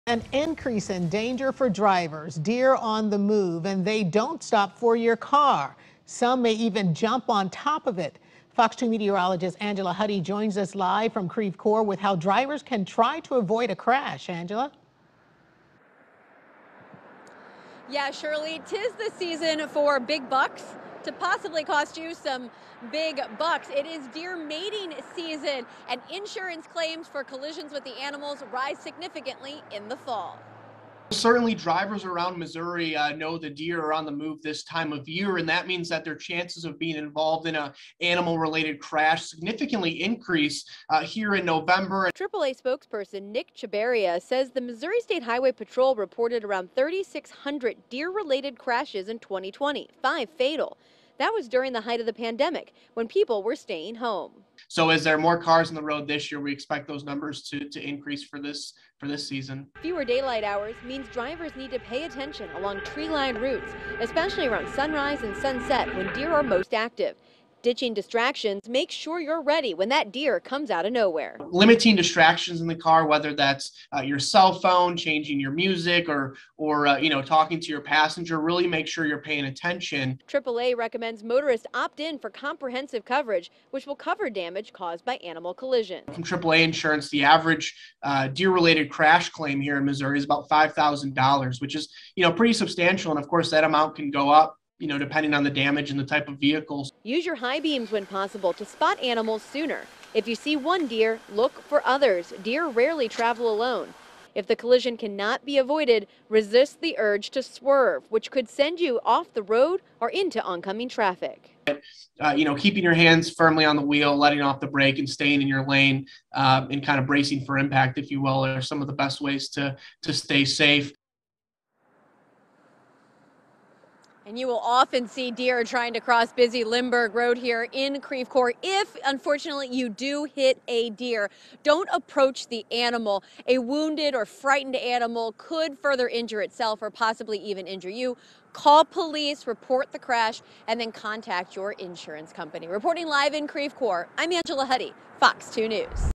Oh deer - AAA says it’s the peak time of year for deer collisions
November is here. ‘Tis the season for big bucks to possibly cost you big bucks. It is deer mating season and insurance claims for collisions rise significantly in the fall.